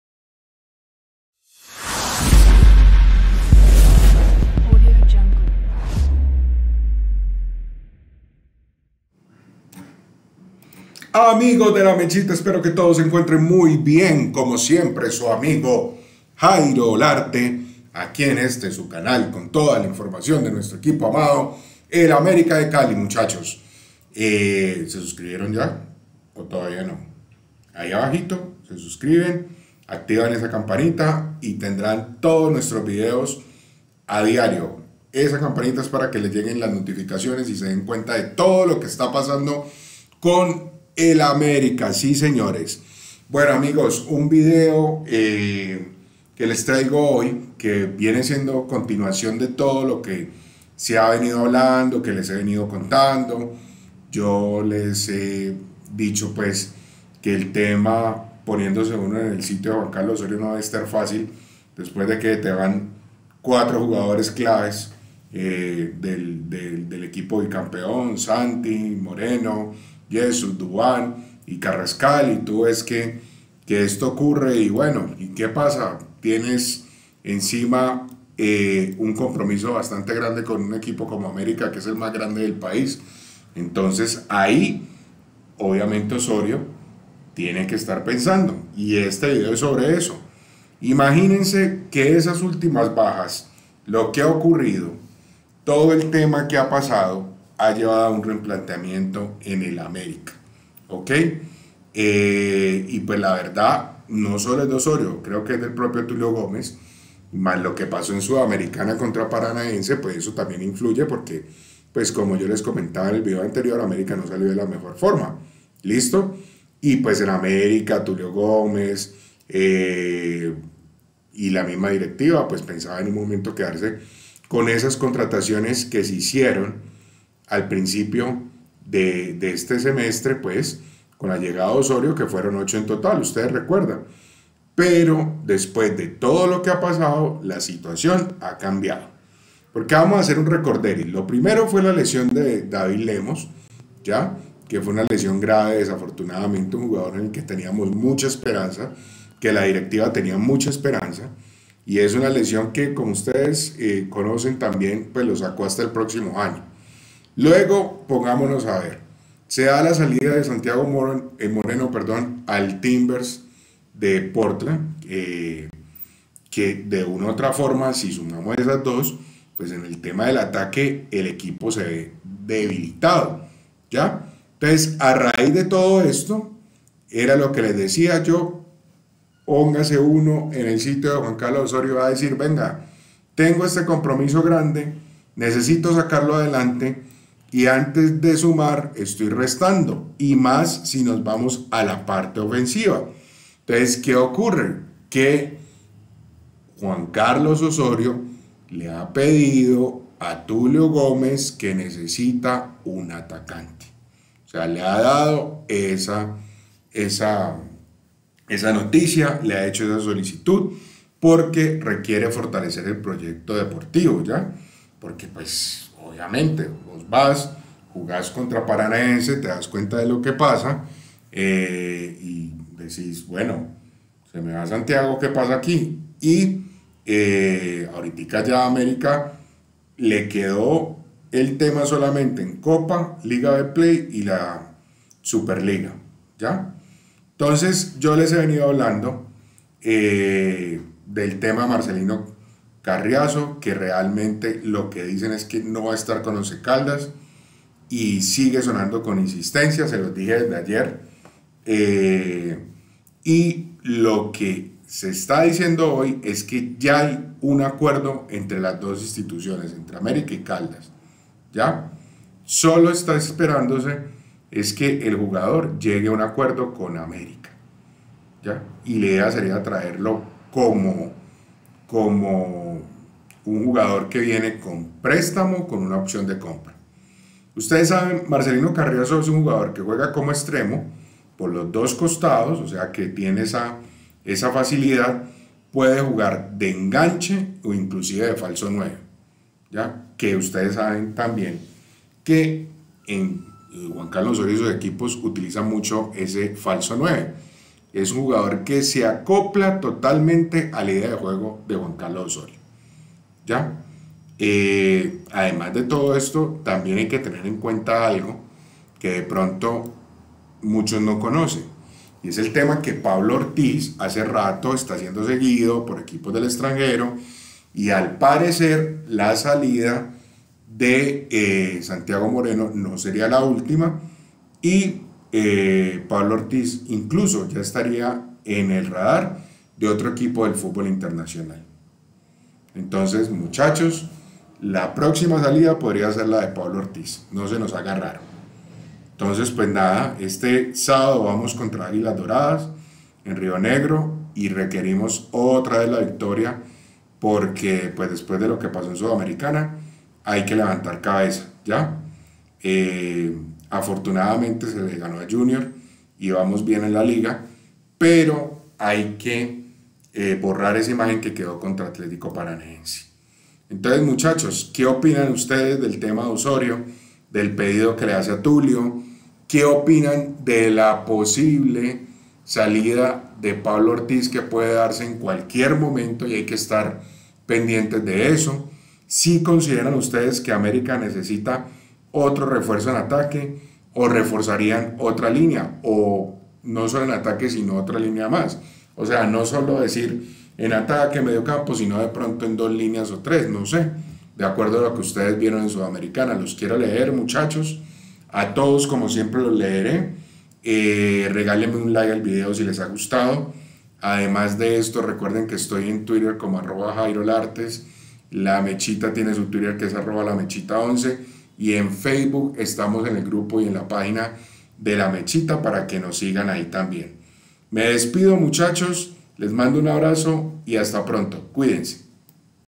🚨EL NUEVO PEDIDO DE OSORIO A TULIO GÒMEZ🚨😱EL PRÒXIMO JUGADOR A SALIR DEL AMÈRICA😱
La realidad de @LaMechita hace que Juan Carlos Osorio le haga un nuevo pedido a Tulio Gómez; la de Santiago Moreno no sería la ultima salida del América.